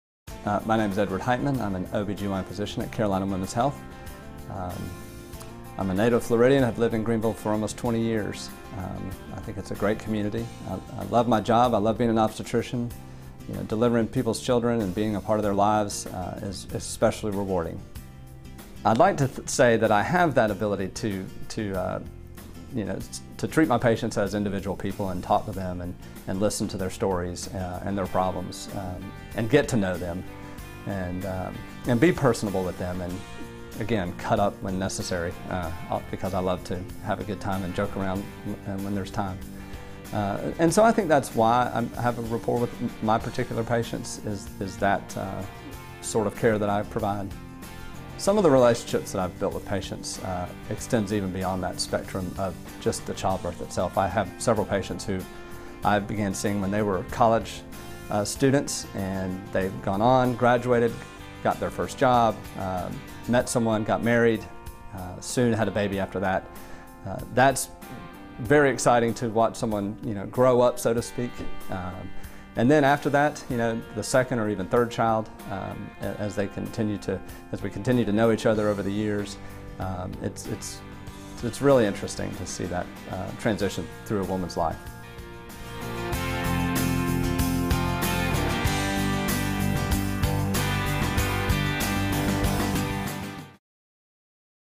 Dr. Edward Heidtman, OB/GYN
Dr. Heidtman is an OB/GYN with Carolina Women's Health in Greenville.